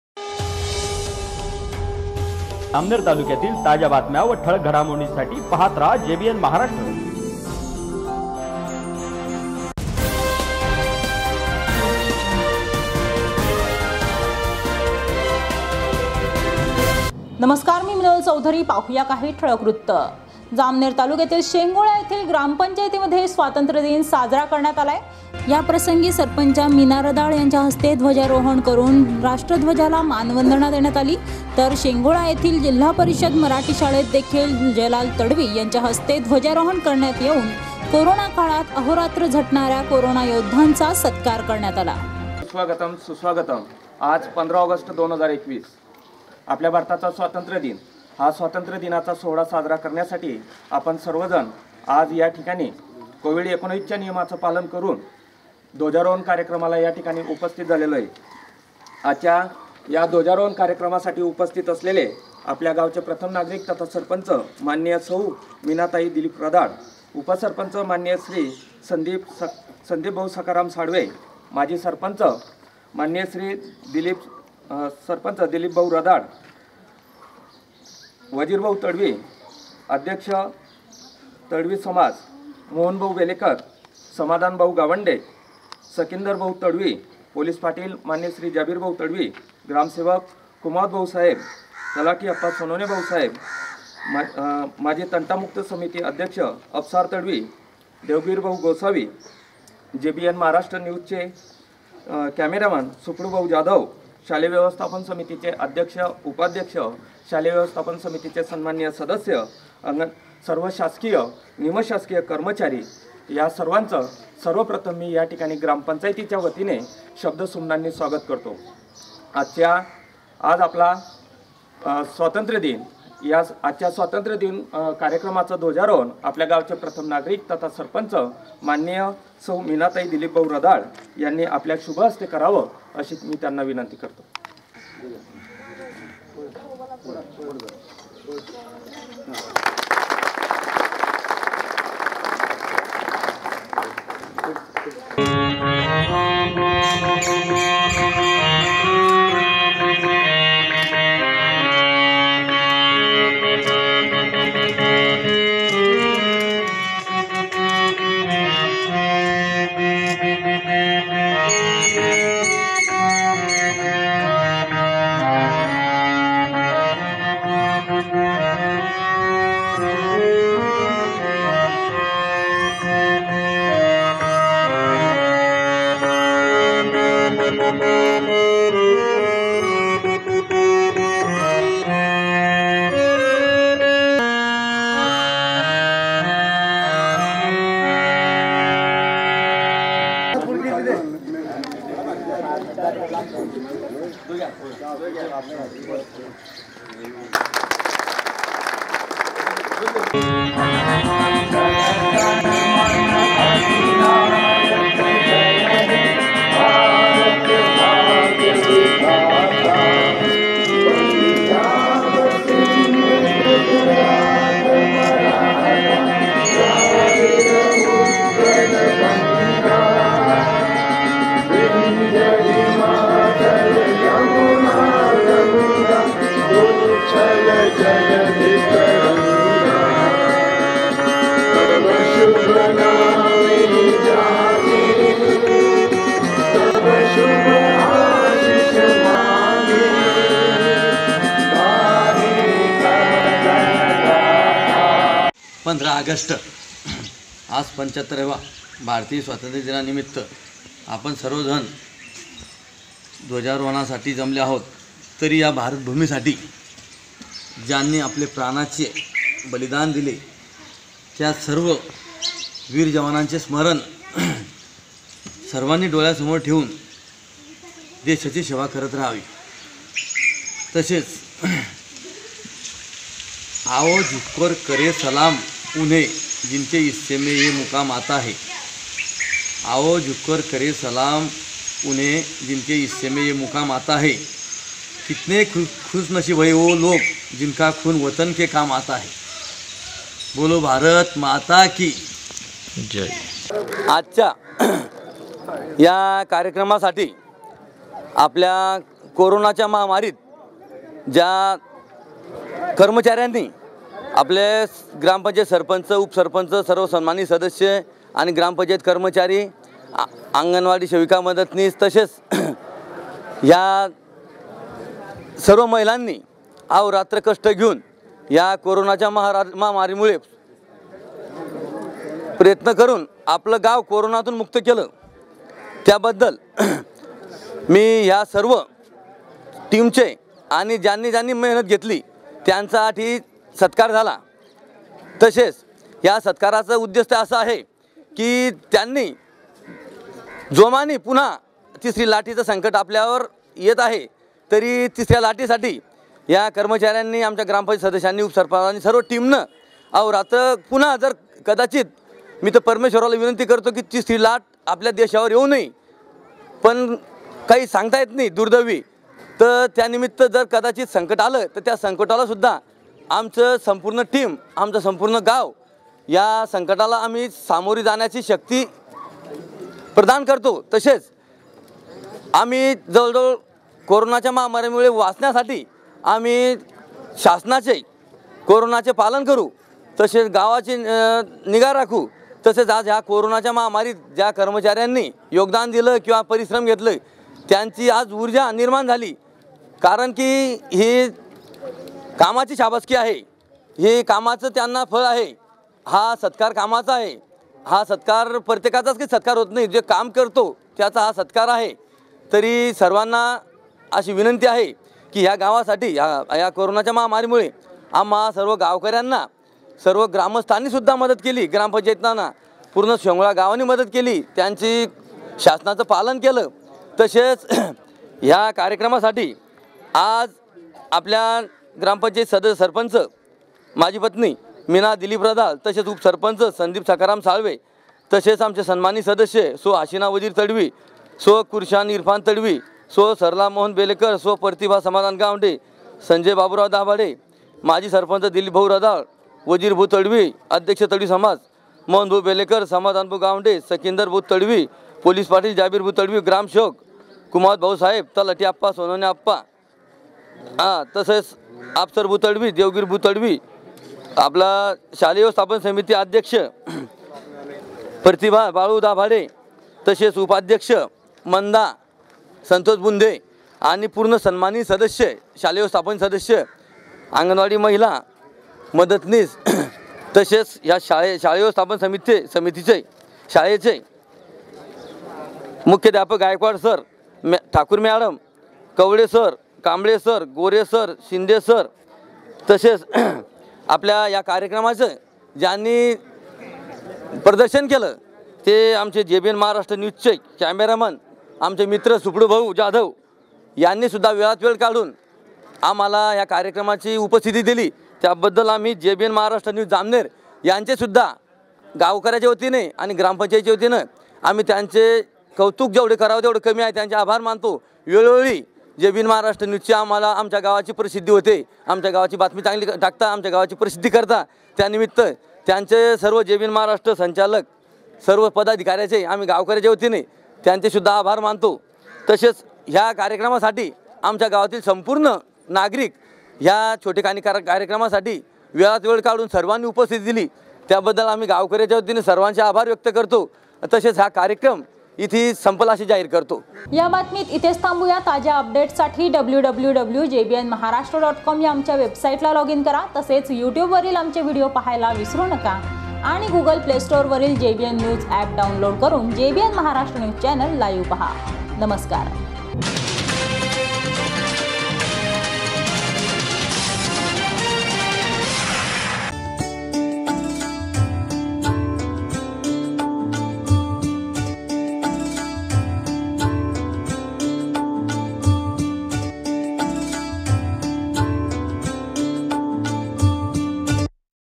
jbn maharashtra news jalgaon
शेंगोळा ग्रामपंचायत येथे सरपंच मीना दिलीप  रडाळ यांच्या हस्ते ध्वजारोहण तर जि.प.शाळेचे ध्वजारोहण वजीर तडवी यांच्या हस्ते करण्यात आले